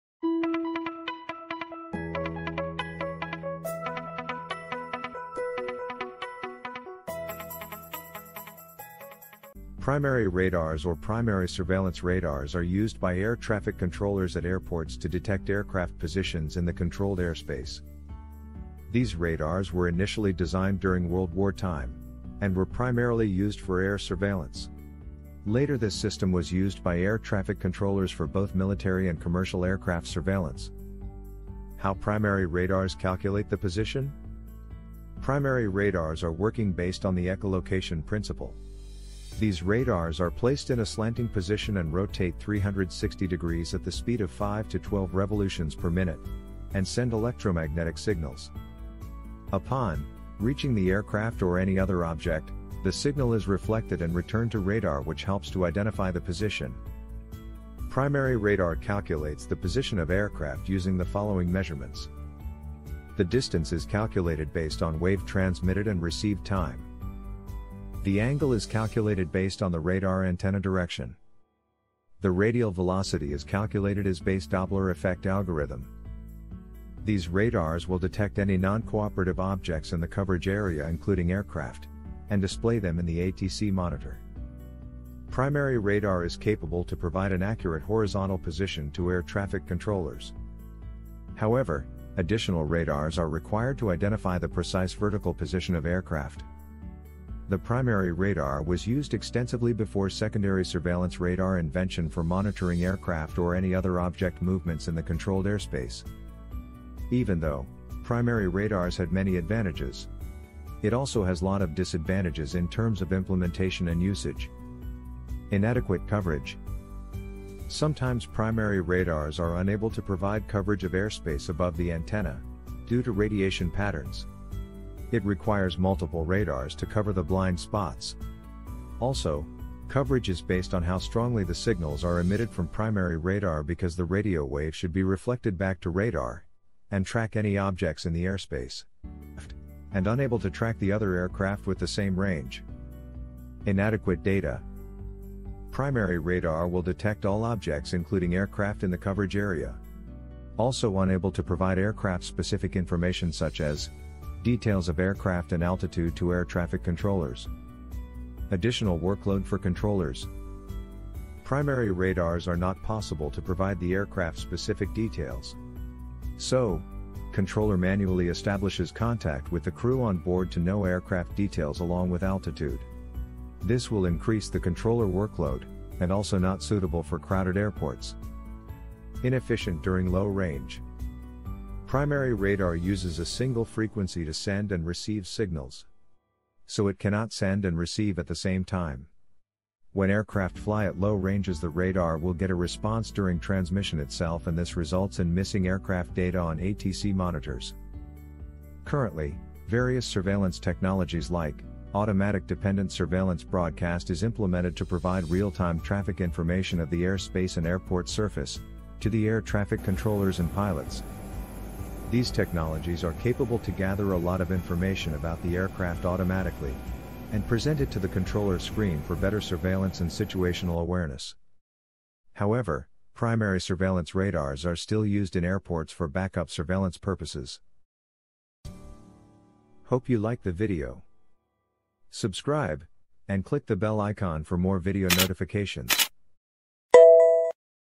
Primary Radar or Primary Surveillance Radar (PSR)
Primary Radar or Primary surveillance radars (PSR) are used by air traffic controllers at airports to detect aircraft positions in the controlled airspace.